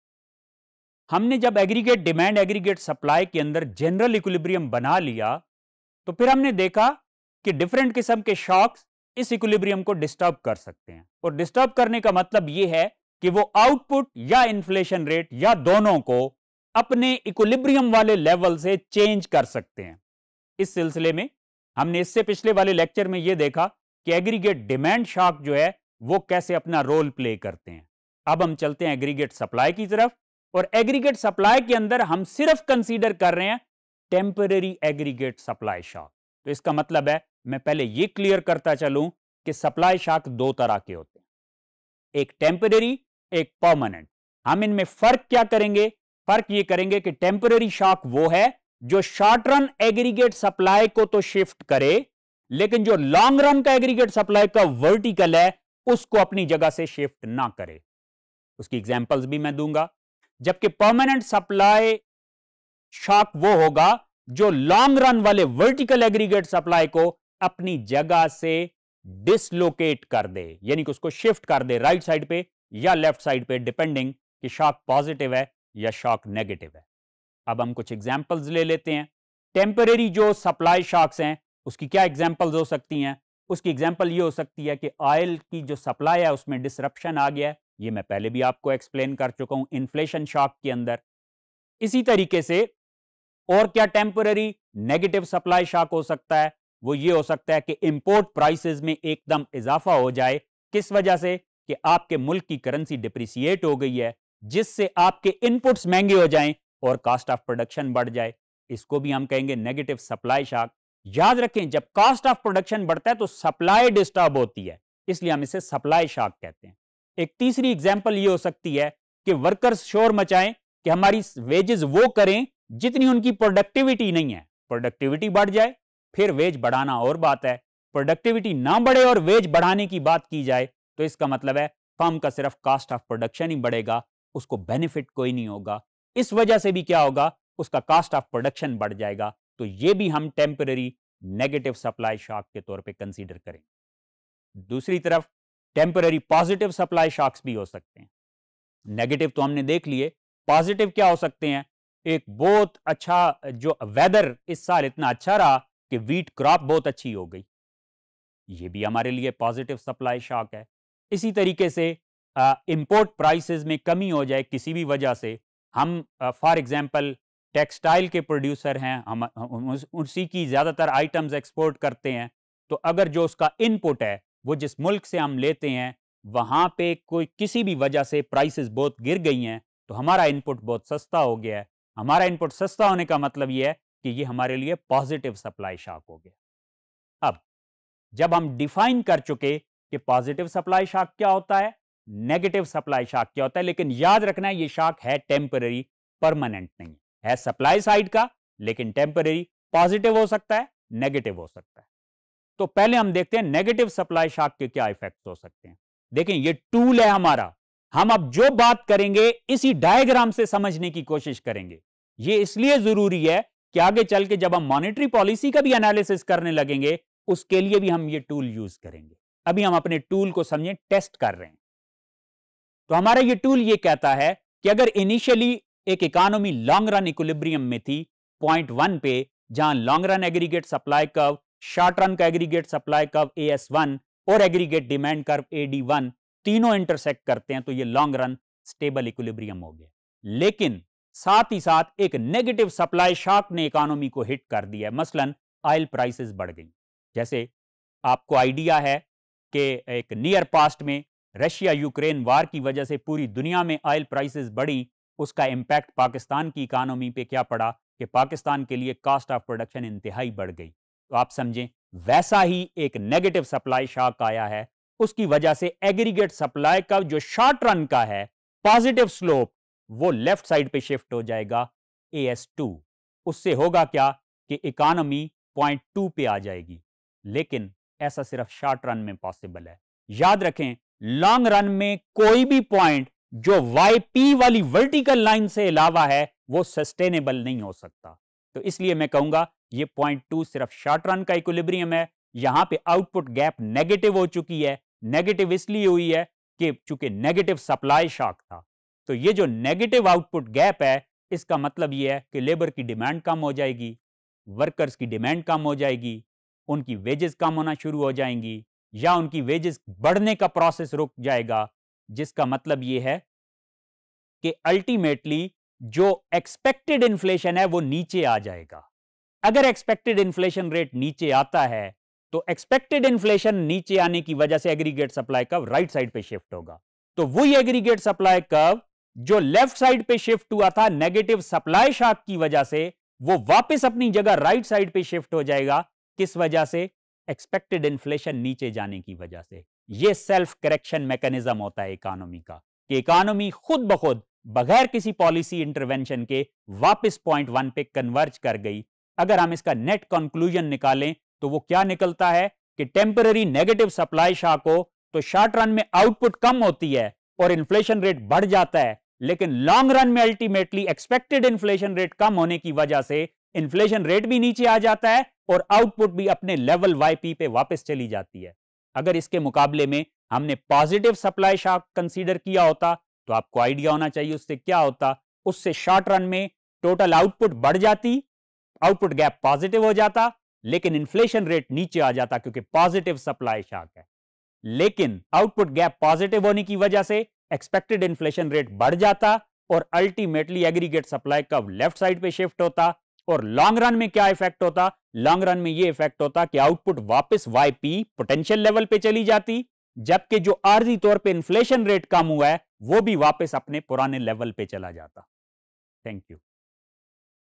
Disturbing Equilibrium in AD and AS – Temporary AS Shocks | Monetary Economics | ECO604_Topic060
Topic060: AD-AS Framework - Disturbing Equilibrium in AD and AS – Temporary AS Shocks,